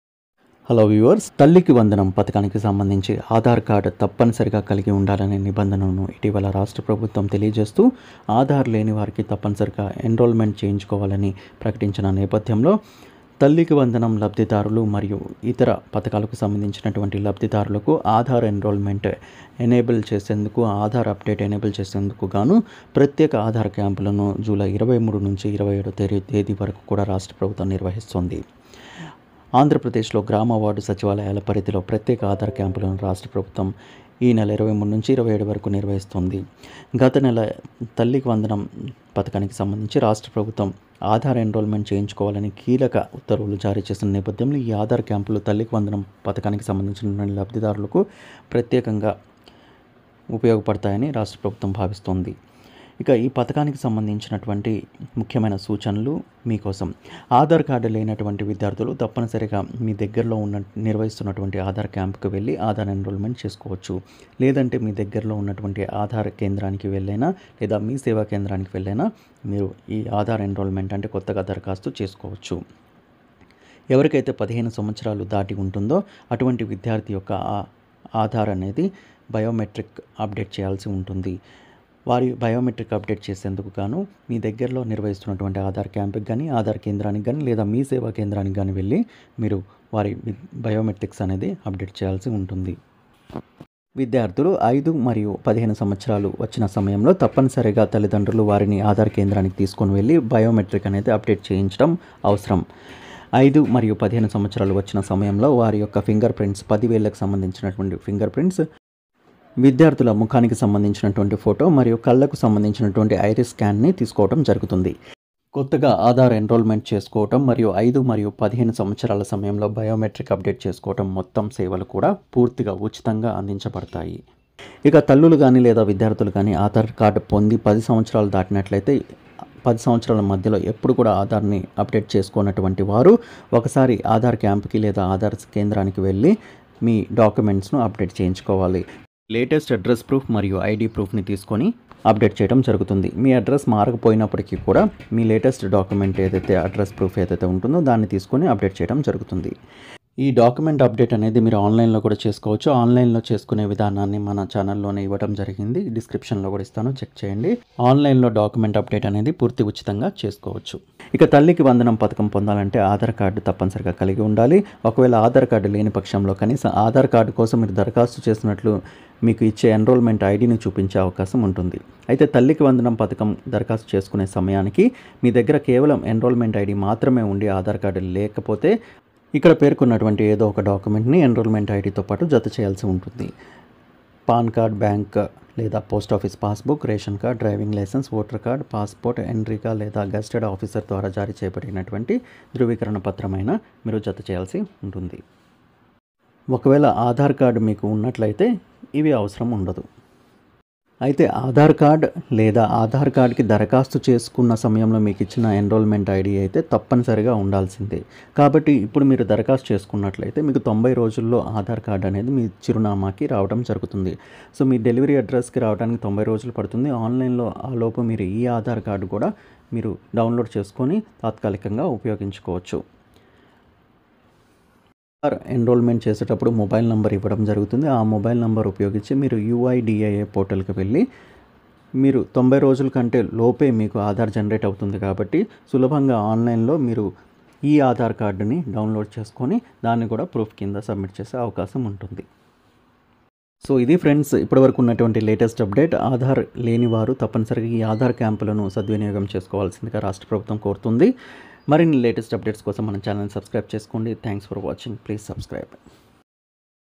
talliki vandanam - aadhar instructions తల్లికి వందనం లబ్ధిదారులకు ఆధార్ క్యాంపులు, ముఖ్య సూచనలు
This video is about aadhar instructions for talliki vandanam beneficiaries

📊 ఆధార్ కార్డు సంబంధించి ముఖ్యమైన లింక్స్